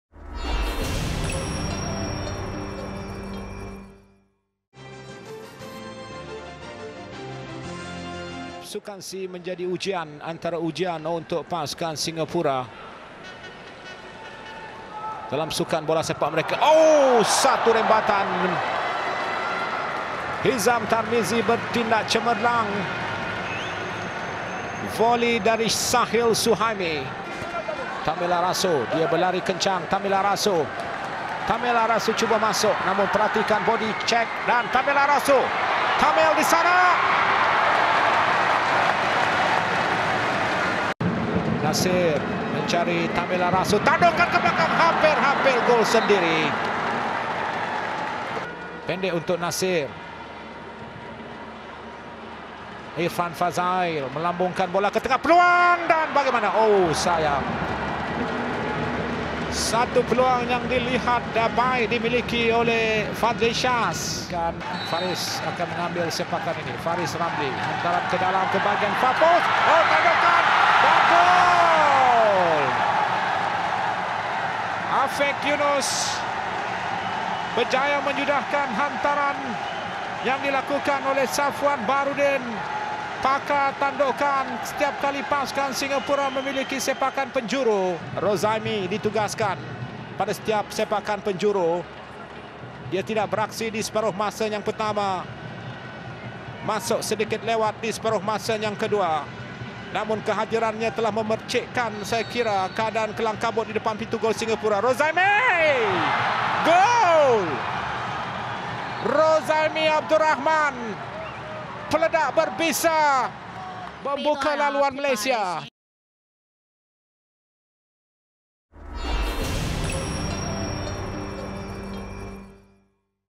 Singapura 1-1 Malaysia (15/12/13)
Perlawanan ketiga buat pasukan bolasepak B-23 tahun kebangsaan di Sukan SEA 2013 Myanmar. Gol lewat dari Rozaimi (90'min)